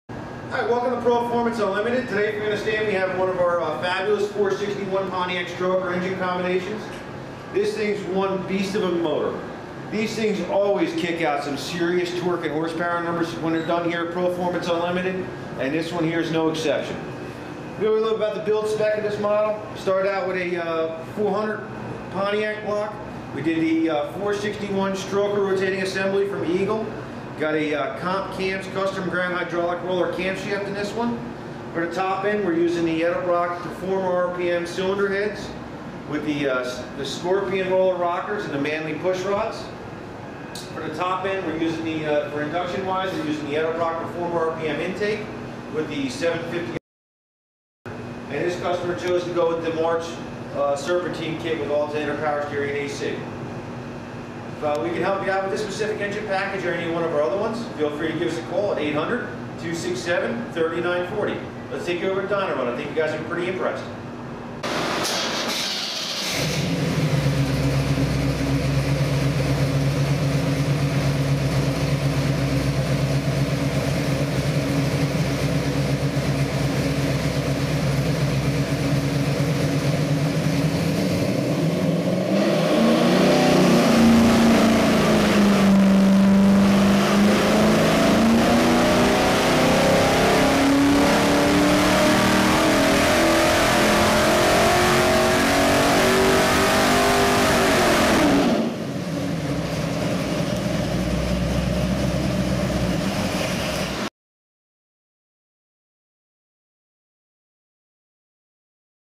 Proformance Unlimited 461 Pontiac Stroker Crate Engine
Proformance Unlimited's Pontiac 461 stroker with 475 HP engine package with Edelbrock aluminum heads and a forged rotating assemble.